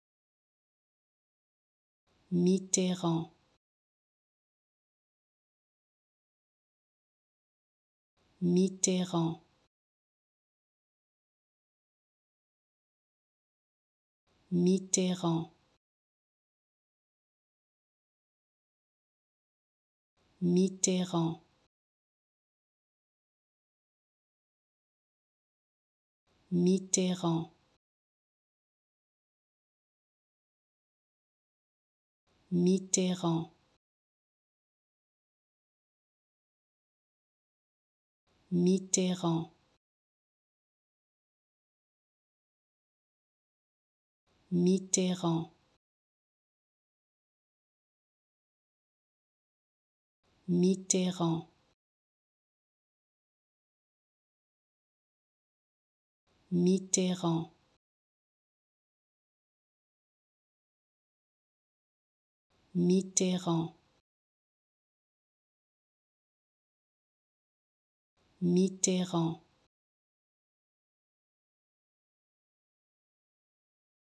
How to Pronounce ''Mitterrand'' Correctly in French
Learn how to say and properly pronounce ''Mitterrand'' in French with this free pronunciation tutorial. Apprenez à prononcer des mots en français grâce à nos tutoriels.